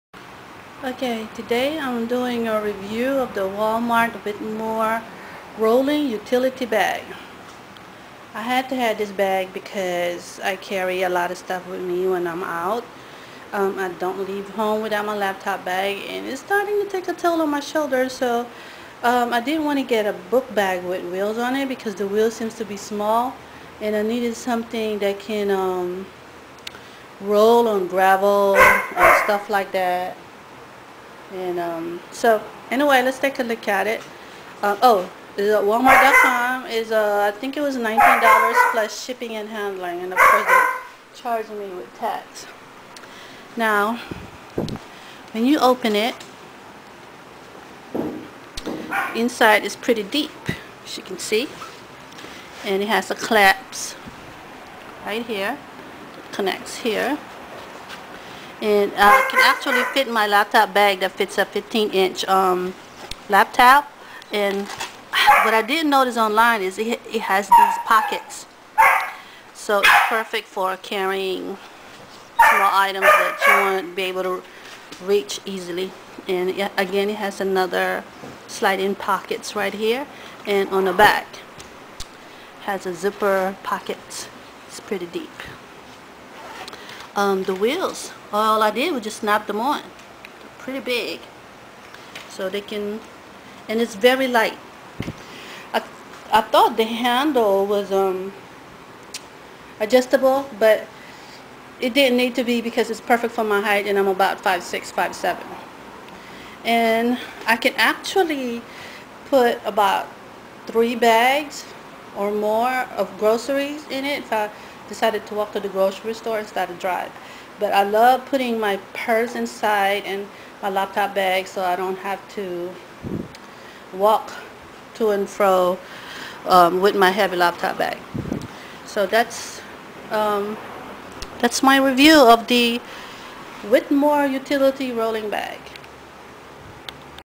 Review of Whitmor Rolling Utility Bag
My shoulders can finally exhale and get back to proper posture.  Thanks to the Whitmor Rolling Utility Bag I purchased from walmart.com for about $20 bucks + shipping and handling.